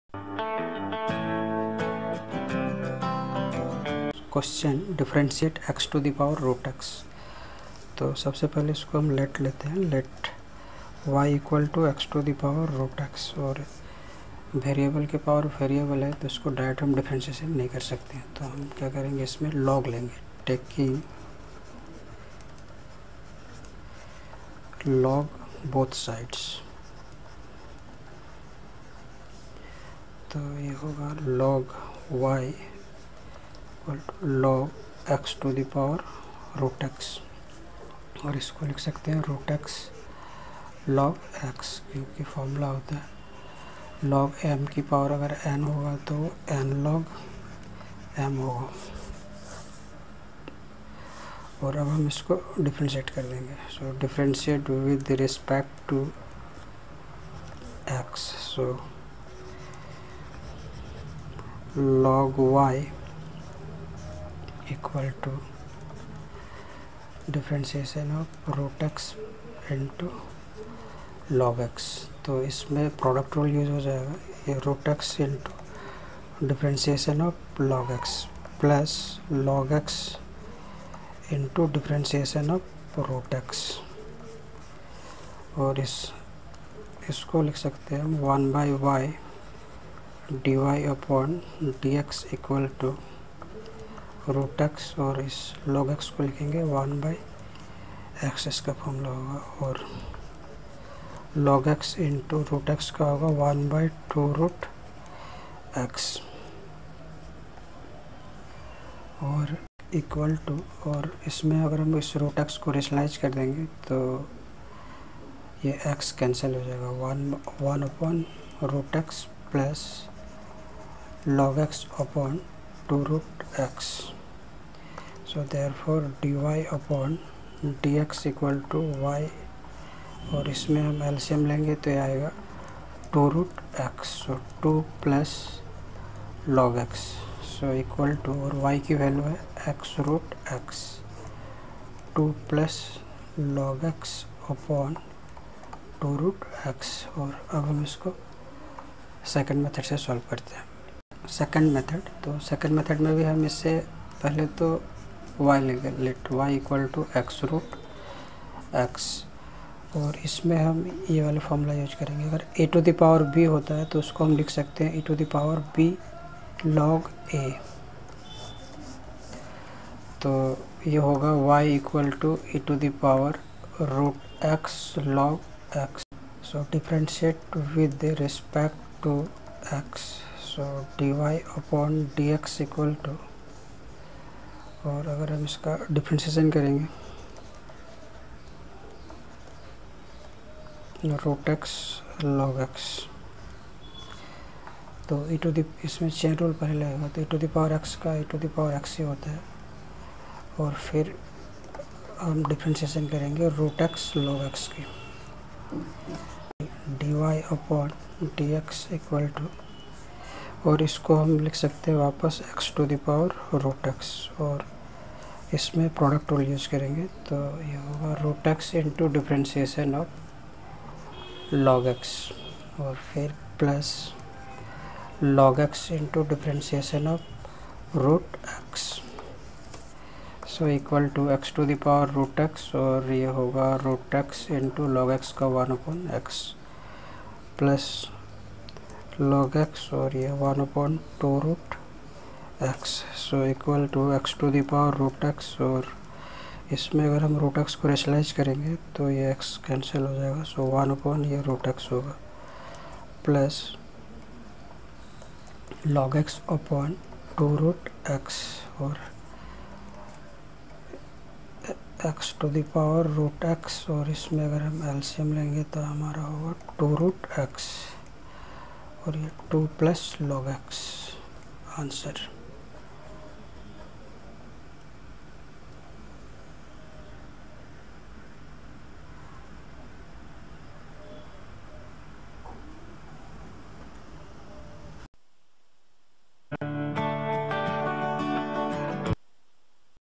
Q2 | Derivative of x^√x | Differentiation of x to the power root x | Logarithmic Differentiation
Q2: Differentiate x^√x with respect to x.